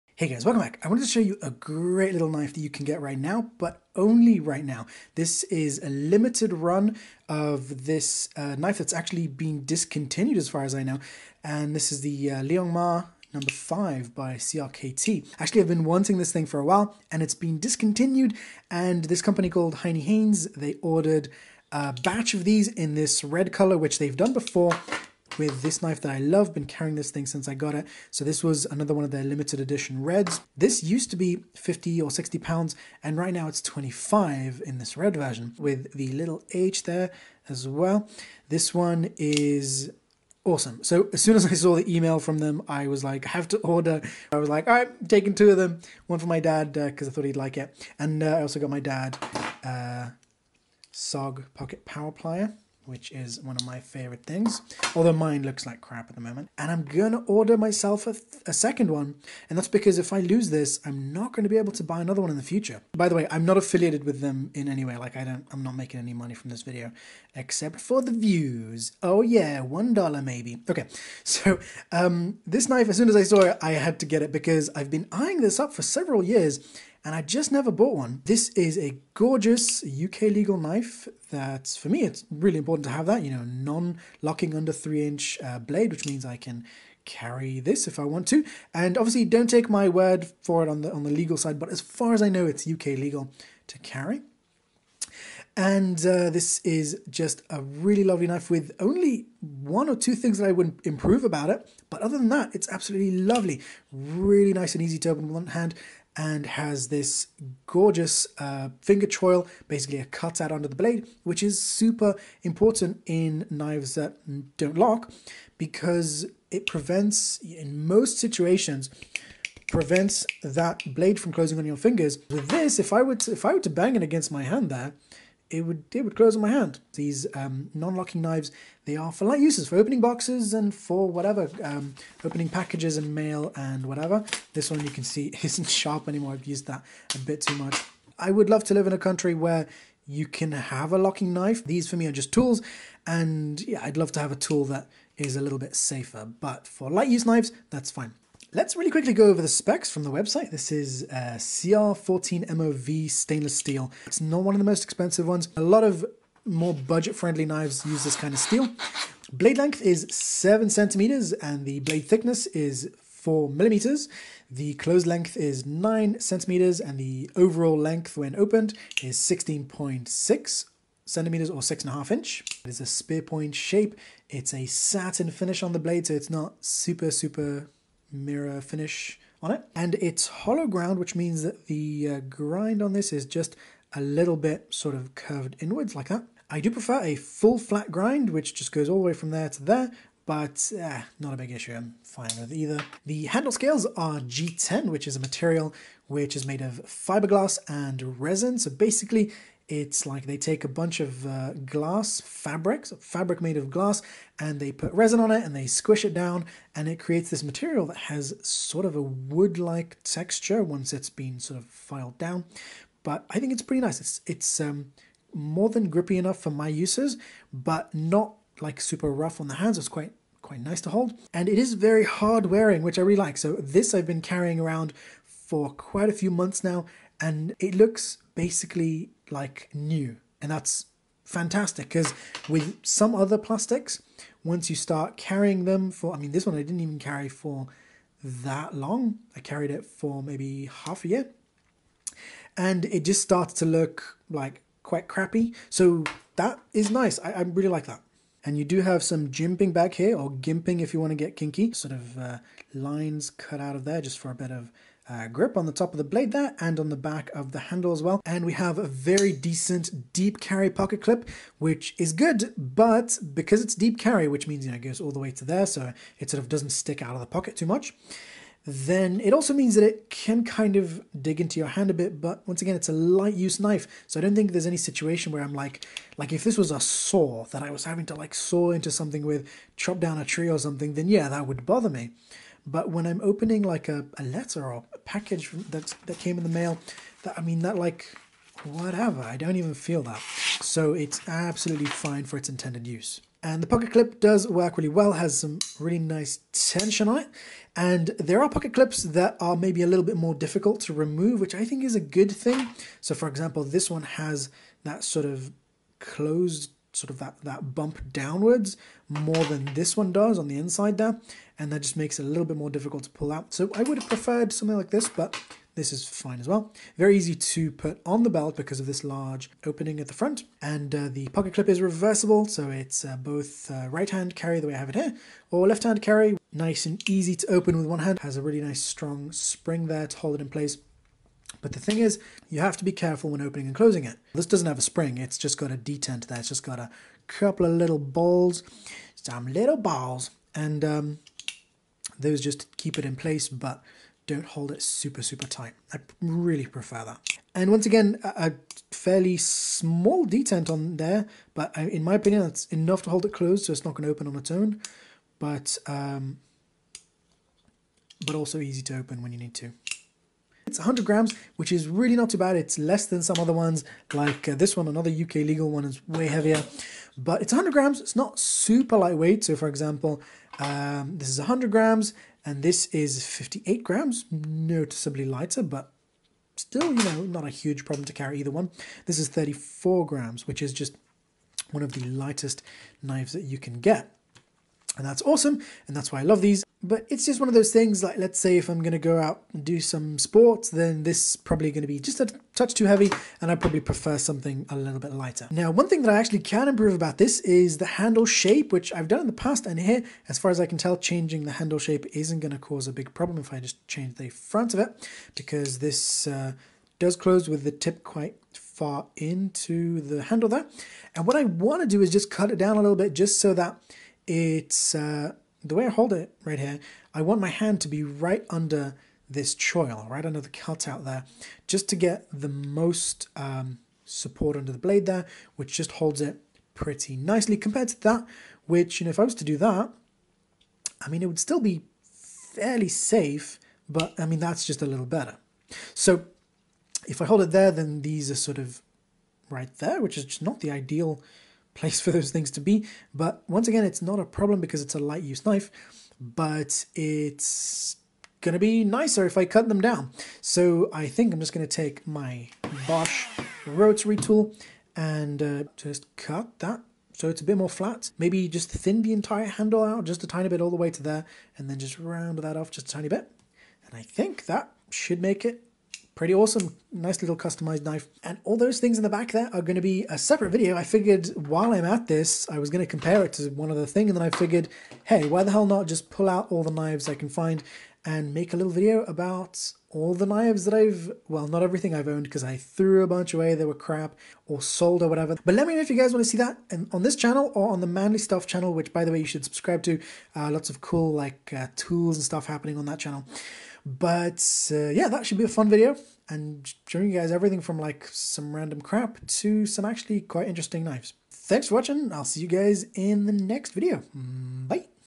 Sweet limited edition UK-legal knife for £25
The first ever episode of Saturday Gadgets! Showing the Heinnie Haynes limited edition CRKT Liong Mah #5 in red G10 scales.